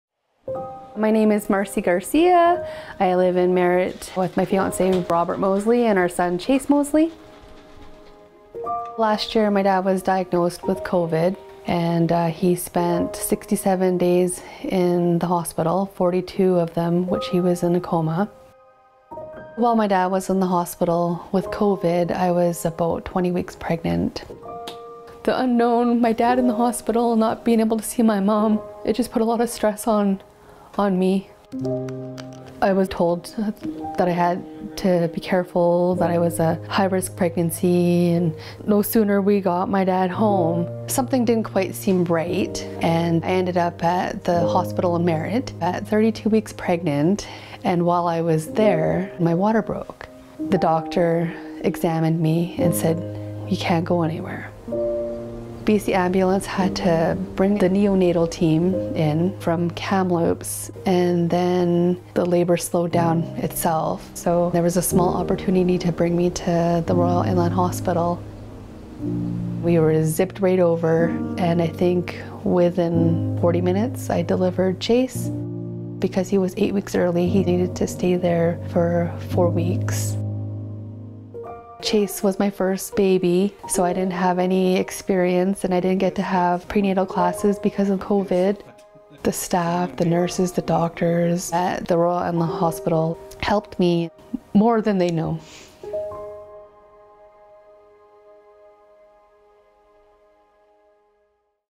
Marcy Garcia | RIH Grateful Patient
"Something didn't quite seem right, and I ended up at the hospital in Merritt at 32 weeks pregnant.  While I was there, my water broke..."

This is the gratefulpatient story of Marcy Garcia, who's high-risk pregnancy and premature labour brought the RIH Neonatal Team all the way from Kamloops to Merritt and back, in order to ensure the safe delivery of her son Chase. You may also recognize Marcy and baby Chase from the @rihfound 'Together We Rise' campaign video we shared recently.

Thank you to all the staff at Royal Inland Hospital and BC Ambulance, who assisted Marcy in bringing baby Chase into this world safely and soundly